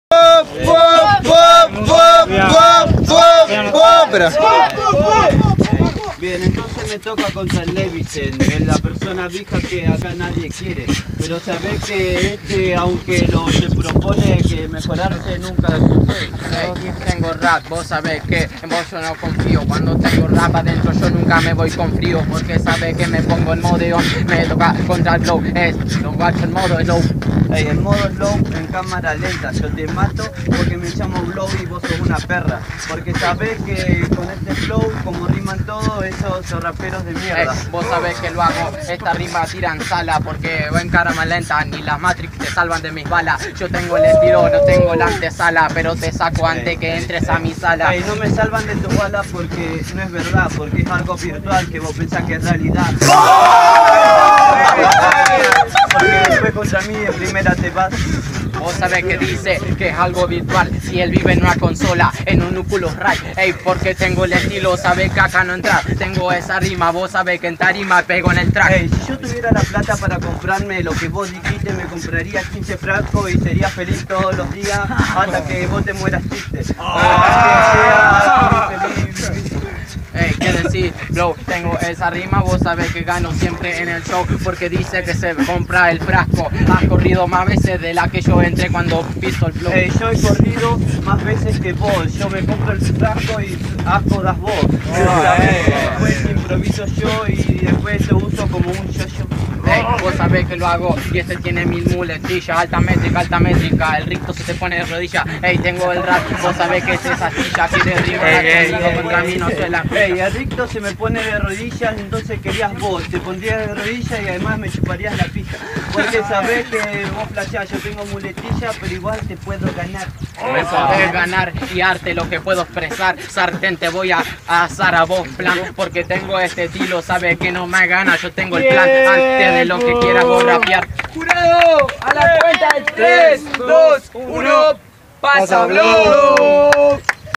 LEVISEN vs BLOW - 8AVOS - RAP CLIQUE 35° - 10° FECHA
Bueno los esperamos a todos el 20 de Diciembre en lo que va a ser la decima fecha de la rap clique en la cual sera por puntos los 7 primeros en la tabla al finalizar el torneo clasificaran a la 2da edicion con micro de rap clique, el que termine primero se llevara un premio sorpresa, espero que puedan caer todos

MODALIDADES:

8vo: 4 x 4 1:20 (sin tematica)

4to: 40 ida y ida (con tematica)

SEMI: 8 x 8 3min (sin tematica)

FINAL: 1min ida y vuelta (con tematica)

GANADOR: BLOW